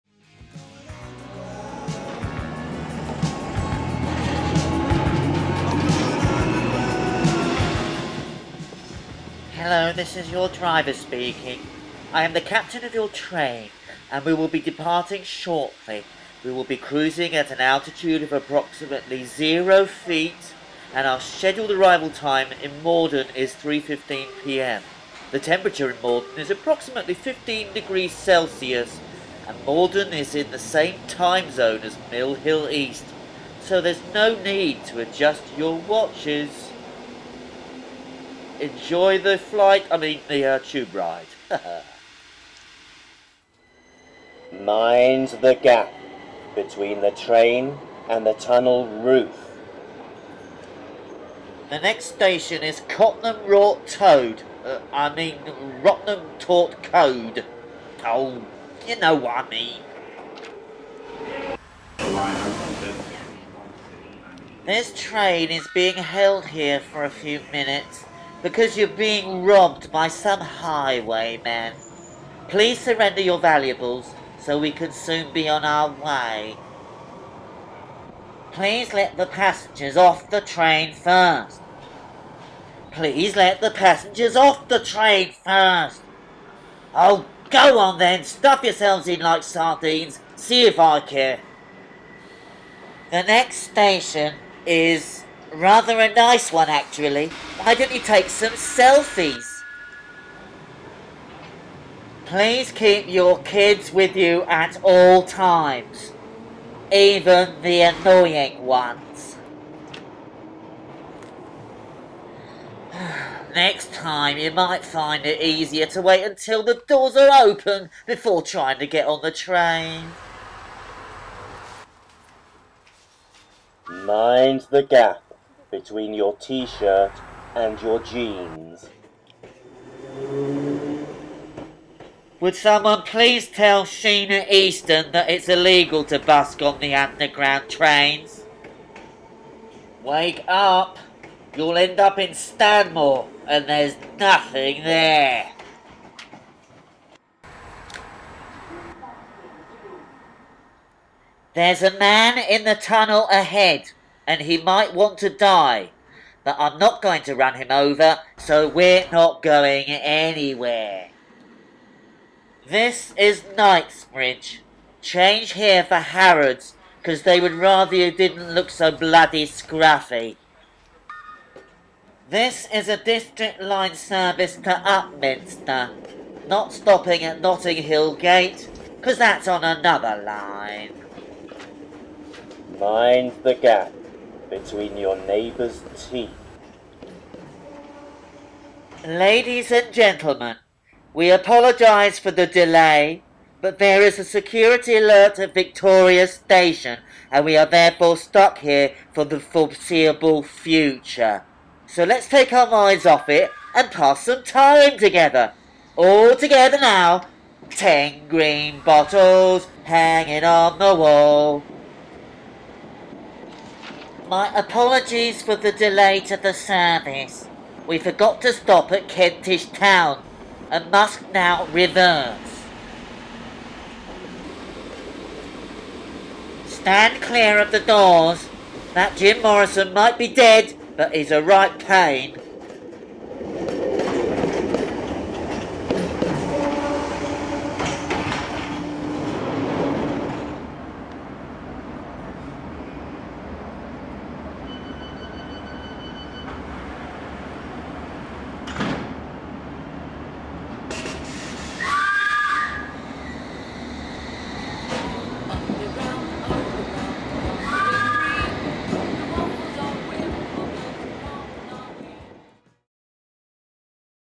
PAUL LINN's FRIDAY FOLLIES: Tube Announcements
Week 24 of my Friday Follies form the office of little work of a Friday. This week is something for London commuters especially, but hopefully everyone will appreciate the fun and silliness involved.